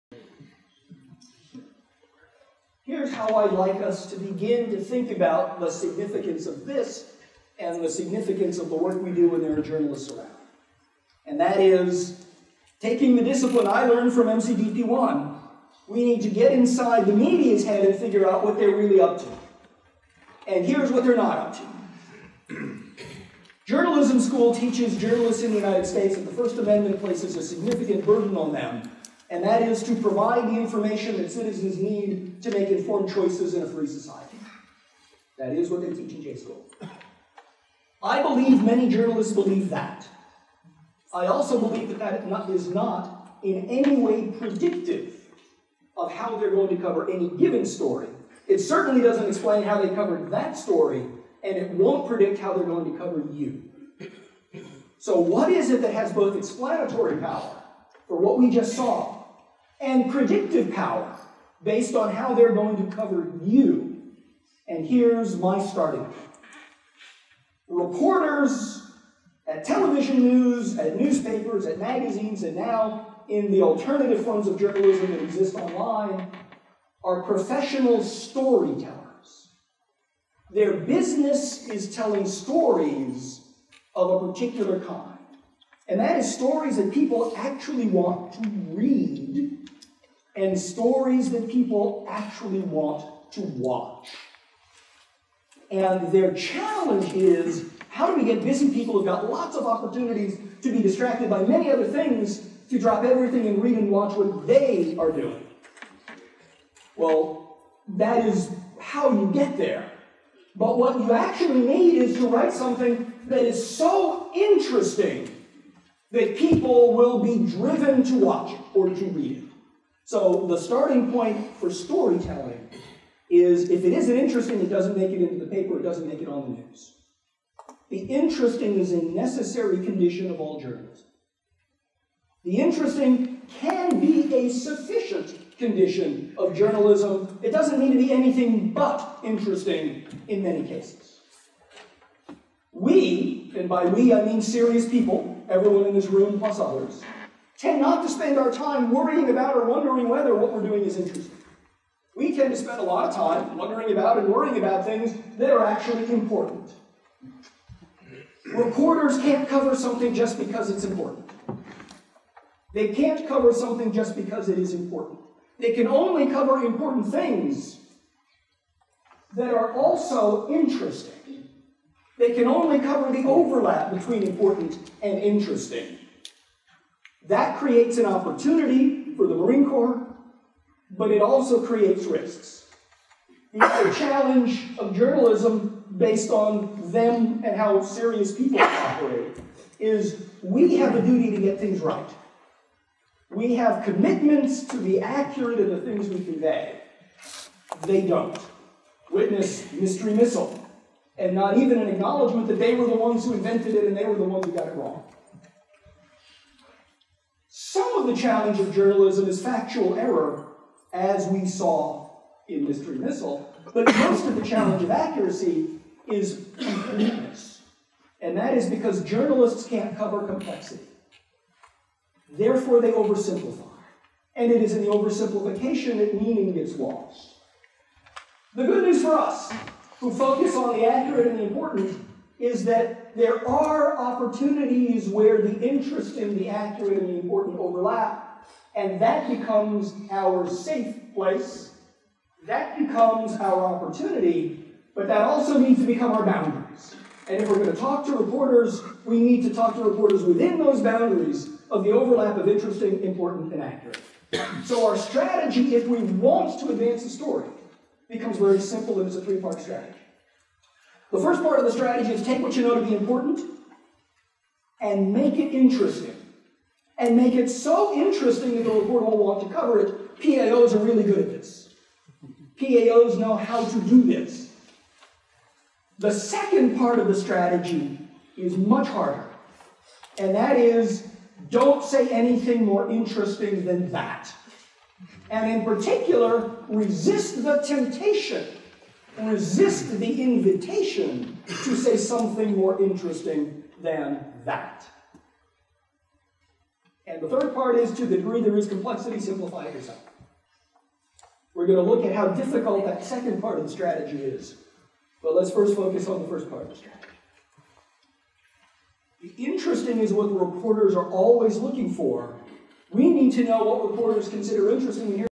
Garcia USMC II MEF Part 3 Reporters are Storytellers
Reporters are Storytellers: Helio Fred Garcia speaking to the leadership of the United States Marine Corps II Marine Expeditionary Force (II MEF) at Camp Lejeune, NC on January 15, 2016.  This was part of a weeklong Professional Military Exercise program for the MEF.  In this excerpt, part 3, Garcia provides a framework for understanding what drives news coverage, setting the stage for the next excerpt, How the News Media Works.  This is an adaptation of the kinds of coaching we provide for our non-military clients.  (Video courtesy of United States Marine Corps, posted with permission.)